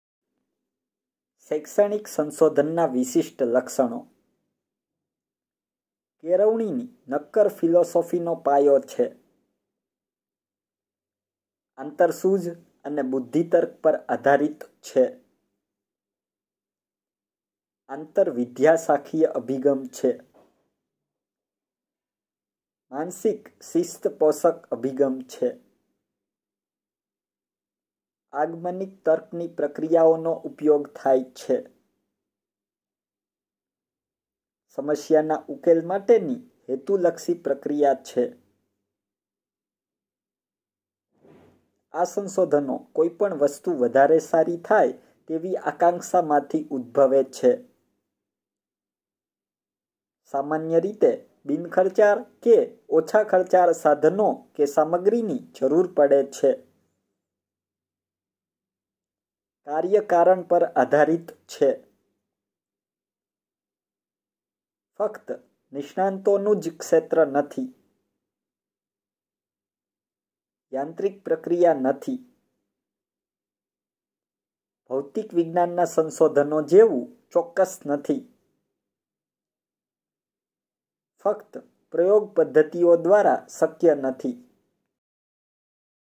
શૈક્ષણિક સંશોધનનું મહત્ત્વ & વિશિષ્ટ લક્ષણો 1.2.4
શૈક્ષણિક સંશોધન...

-કેળવણીની નક્કર ફિલોસોફીનો પાયો છે.
-આંતરસૂઝ & બુધ્ધિતર્ક પર આધારિત છે.
-આંતરવિદ્યાશાખીય અભિગમ છે.
-માનસિક શિસ્તપોષક અભિગમ છે.
-આગમનિક તર્કની પ્રક્રિયાઓનો ઉપયોગ થાય છે.
-સમસ્યાના ઉકેલ માટેની હેતુલક્ષી પ્રક્રિયા છે.
-કોઈ પણ વસ્તુ વધારે સારી થાય તેવી આકાંક્ષામાંથી ઉદ્દભવે છે.
-સામાન્ય રીતે બિનખર્ચાળ કે ઓછા ખર્ચાળ સાધનો કે -સામગ્રીની જરૂર પડે છે.
-કાર્યકારણ પર આધારિત છે.
-ફક્ત નિષ્ણાંતોનું ક્ષેત્ર નથી.
-યાંત્રિક પ્રક્રિયા નથી.
-ભૌતિકવિજ્ઞાનનાં સંશોધનો જેવું ચોક્કસ નથી.
-ફક્ત પ્રયોગ પદ્ધતિઓ દ્વારા શક્ય નથી.